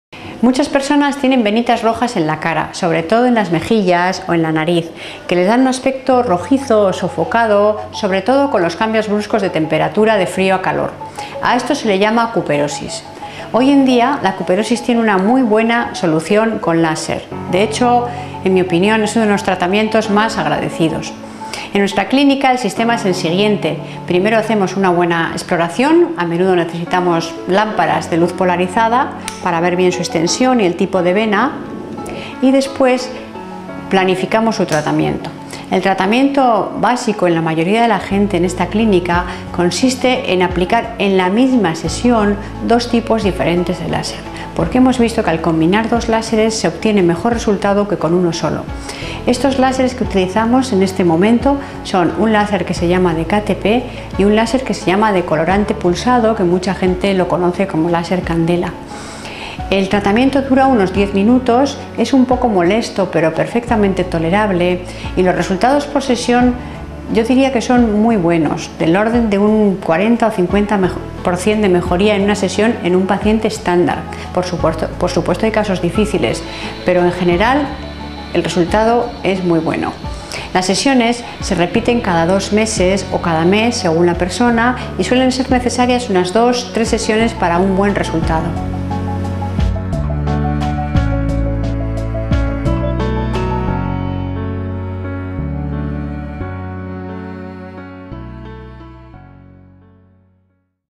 ¿Qué es y cómo se trata la cuperosis? Venitas rojas en la cara | Clínica Dermitek Bilbao
La dra. Landa, co-directora y dermatóloga de la Clínica Dermitek, nos explica qué es la cuperosis, cómo se trata en la clínica y qué resultados se obtienen.

Nuestra dirección:
Paseo de Uribitarte 11-12
48001, Bilbao, Bizkaia